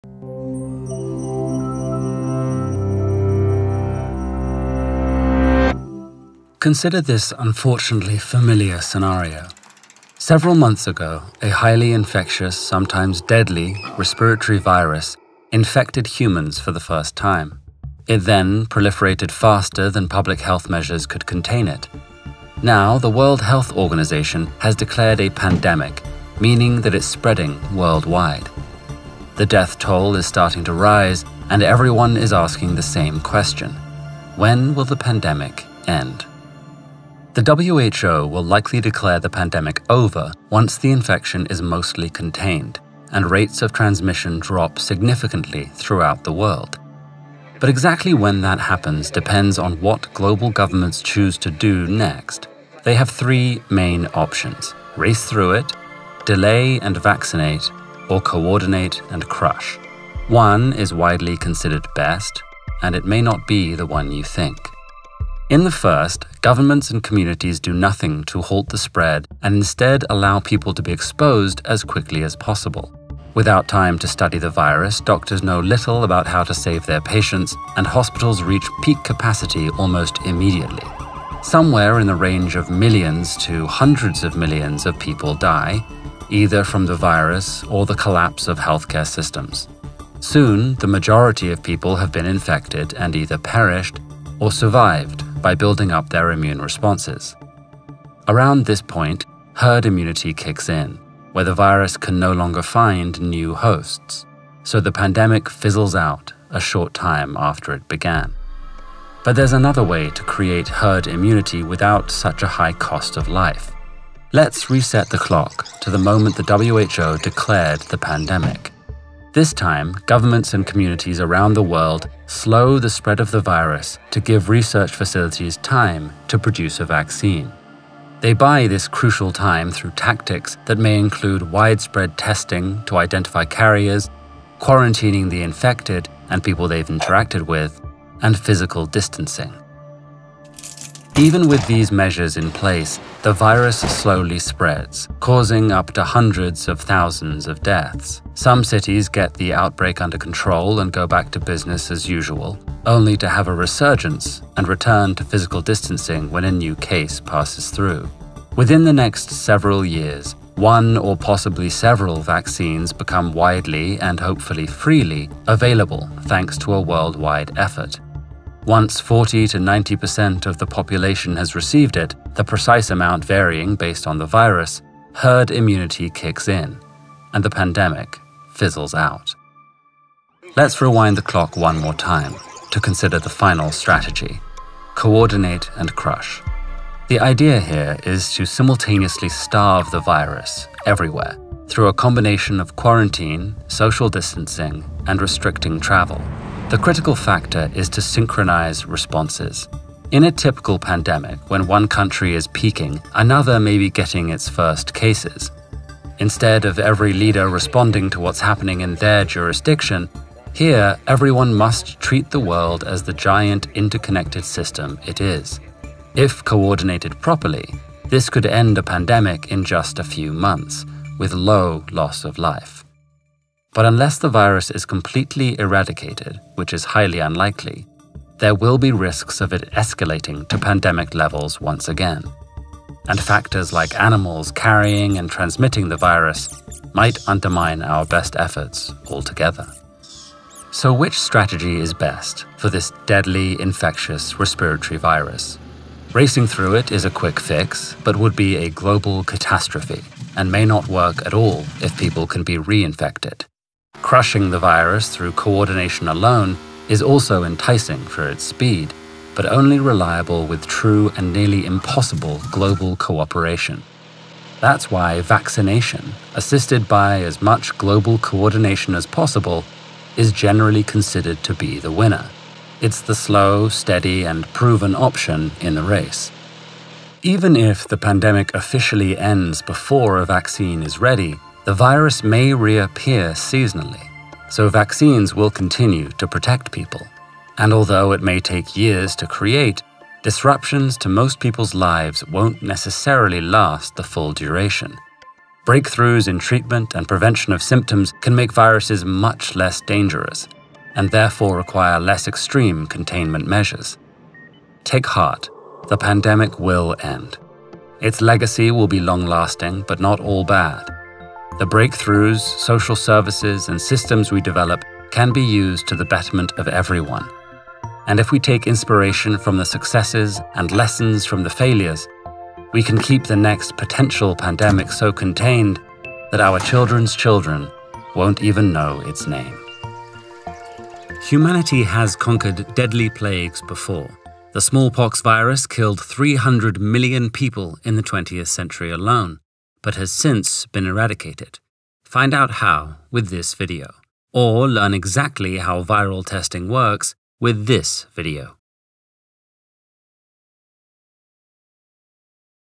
When is a pandemic over?
Explore the three strategies governments and communities can use to contain and end a pandemic, and find out which method is best.

Consider the following scenario: a highly infectious, sometimes deadly respiratory virus infects humans for the first time. It spreads rapidly worldwide, and the WHO declares a pandemic. The death toll starts to rise and everyone is asking the same question: when will the pandemic end? Alex Rosenthal details the three main strategies governments can use to contain and end a pandemic.

Lesson by Alex Rosenthal, directed by Visorama.

Thank you so much to our patrons for your support! Without you this video would not be possible!